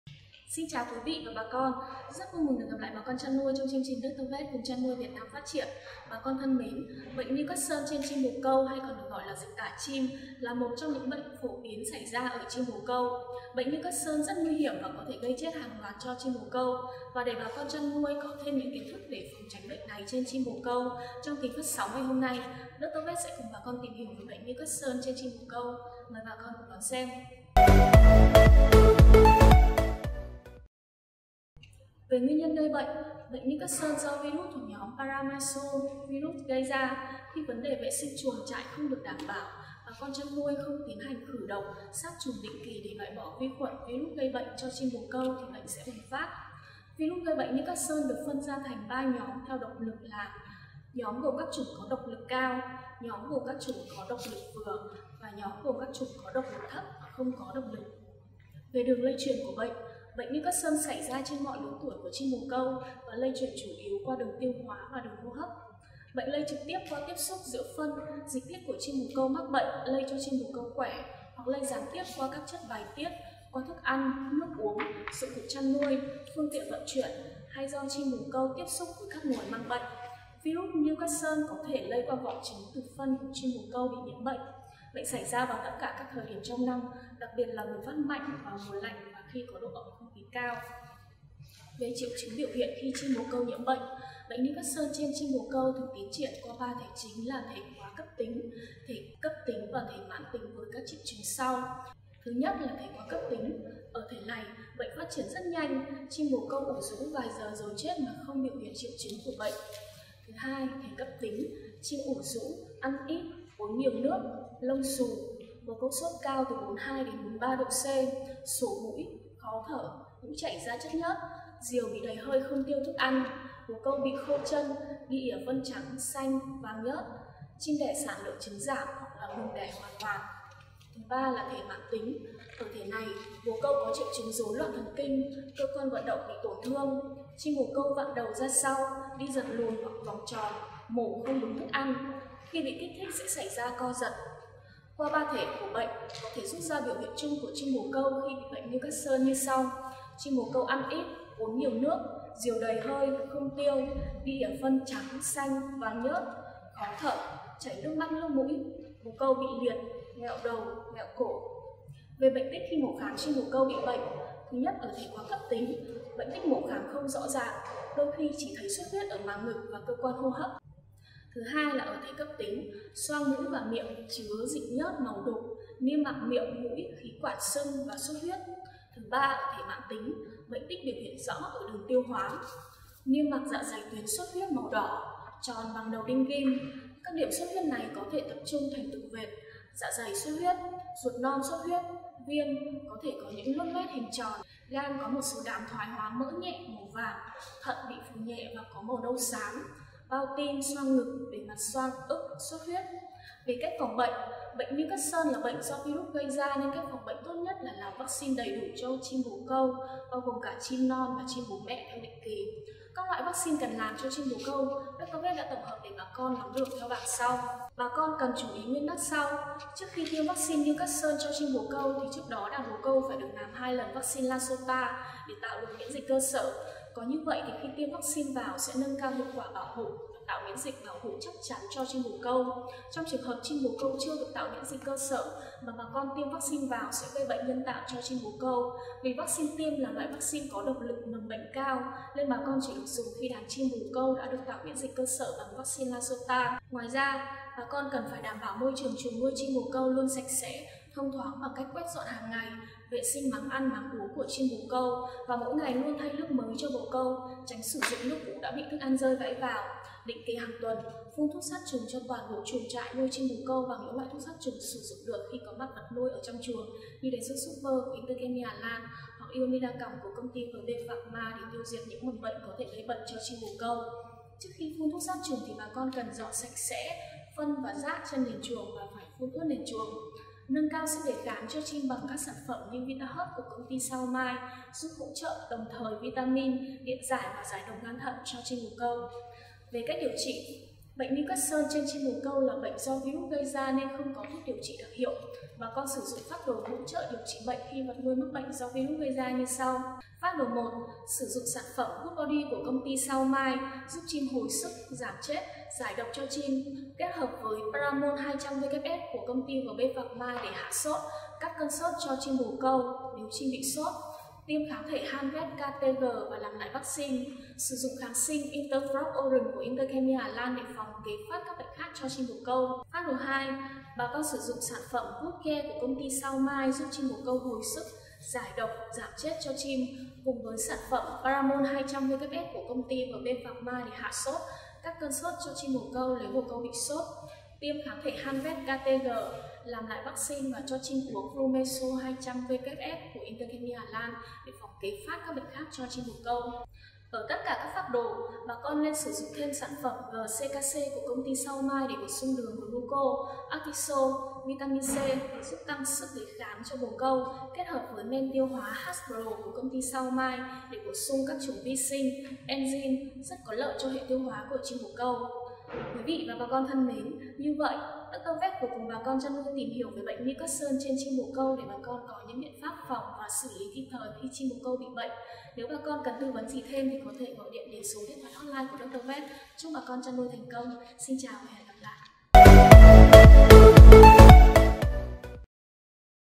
Bệnh Newcastle trên chim bồ câu và cách xử lý hiệu quả | Dr.Vet
BỆNH NEWCASTLE TRÊN CHIM BỒ CÂU VÀ CÁCH XỬ LÝ HIỆU QUẢ
Bệnh Newcastle là bệnh nguy hiểm do virus thuộc nhóm Paramyxovirus gây bệnh cho chim bồ câu ở mọi lứa tuổi, đặc trưng bởi hiện tượng xuất huyết, viêm loét đường tiêu hoá. Bệnh lây lan nhanh gây thiệt hại nghiêm trọng, có thể đến 100% trên đàn chim bị bệnh. Mời bà con tham khảo chuyên đề " Bệnh Newcastle trên chim bồ câu và cách xử lý hiệu quả"
o0o
0:00 - 0:30 Mở đầu
0:31 - 0:58 Nguyên nhân gây bệnh
0:59 - 1:30 Đường lây truyền của bệnh
1:31 - 2:42 Triệu trứng biểu hiện của bệnh
2:43 - 03:23 Bệnh tích khi mổ khám chim bị bệnh
03:24 - 04:49 Cách phòng bệnh
04:50 - 06:26 Cách điều trị bệnh